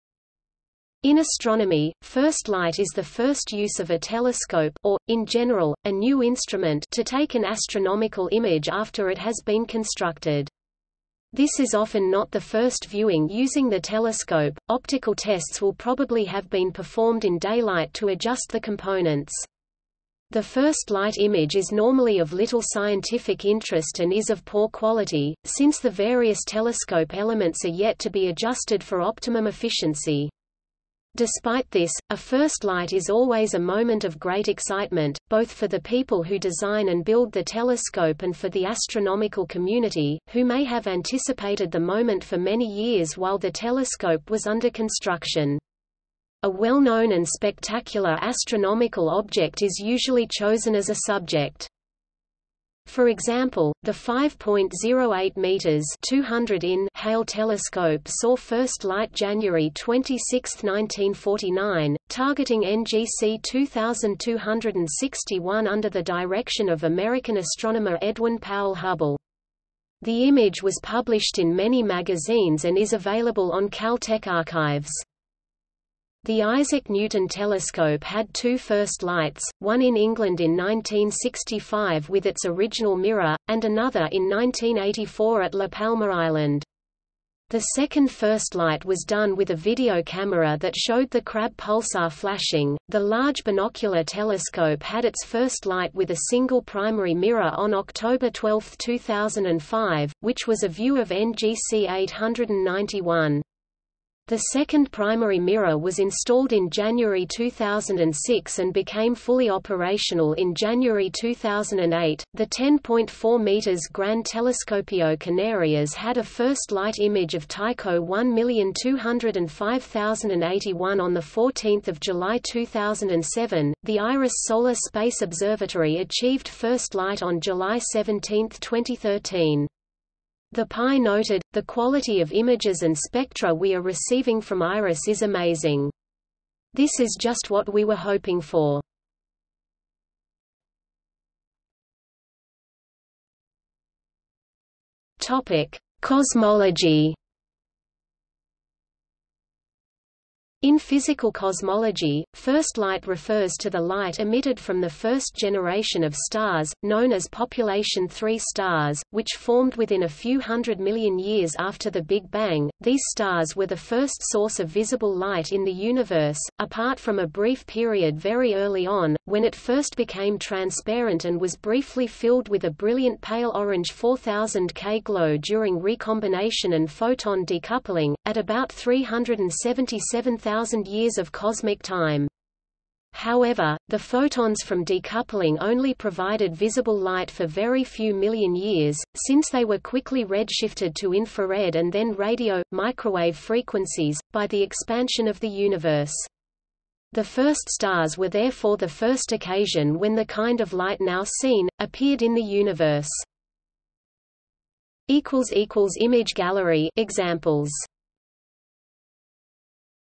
First light (astronomy) | Wikipedia audio article
00:02:21 Cosmology

- Socrates

SUMMARY
In astronomy, first light is the first use of a telescope (or, in general, a new instrument) to take an astronomical image after it has been constructed. This is often not the first viewing using the telescope; optical tests will probably have been performed in daylight to adjust the components. The first light image is normally of little scientific interest and is of poor quality, since the various telescope elements are yet to be adjusted for optimum efficiency. Despite this, a first light is always a moment of great excitement, both for the people who design and build the telescope and for the astronomical community, who may have anticipated the moment for many years while the telescope was under construction. A well-known and spectacular astronomical object is usually chosen as a subject.
For example, the 5.08-metre (200 in) Hale Telescope saw first light January 26, 1949, targeting NGC 2261 under the direction of American astronomer Edwin Powell Hubble.  The image was published in many magazines and is available on Caltech Archives.
The Isaac Newton Telescope had two first lights: one in England in 1965 with its original mirror, and another in 1984 at La Palma island. The second first light was done with a video camera that showed the Crab Pulsar flashing.The Large Binocular Telescope had its first light with a single primary mirror on October 12, 2005, which was a view of NGC 891. The second primary mirror was installed in January 2006 and became fully operational in January 2008.The 10.4 m Gran Telescopio Canarias had a first light image of Tycho 1205081 on 14 July 2007.The IRIS solar space observatory achieved first light on July 17, 2013. The PI noted, "The quality of images and spectra we are receiving from IRIS is amazing. This is just what we were hoping for ..."